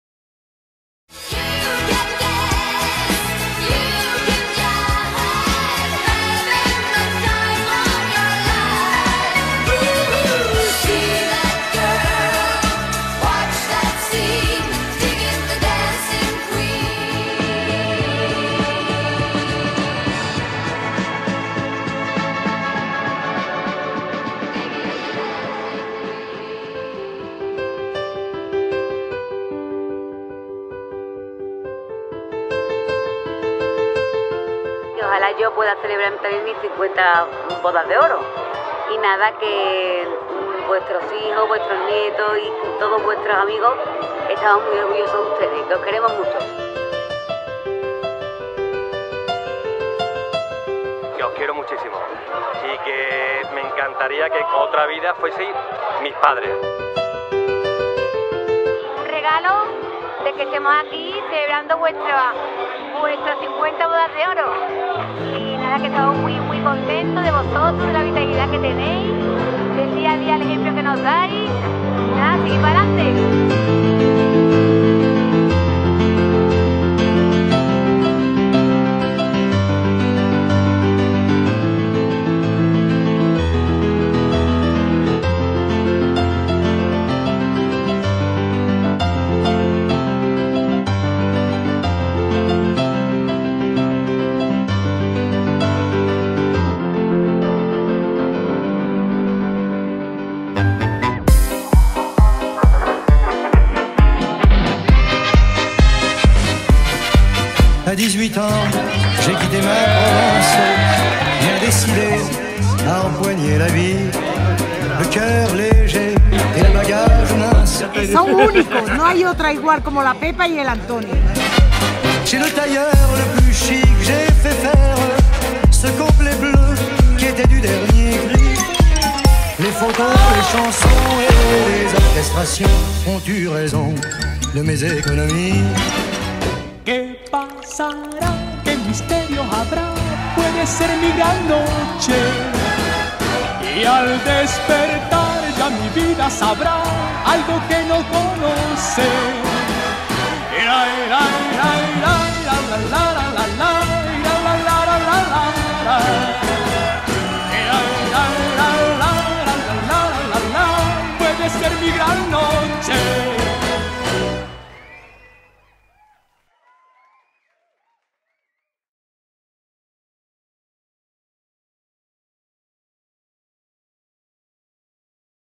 BODAS DE ORO. Pepa + Antonio
🎂🎂🎂50 bodas de Oro🎂🎂en la Capilla de la Trianera y celebración en Abades Triana.
Despues de toda una vida juntos, siguen mirandose con complicidad. Es sencillamente maravilloso.🥇

Vestido Novia: Fernando Claro
Ramo Novia: Las flores de Lucía
Catering: Abades Triana
Calzado Novia: Membur
Traje Novio: Protocolo
Makeup: Noelia Díaz
Fotografía: Oskydearte
Video: Panchete Films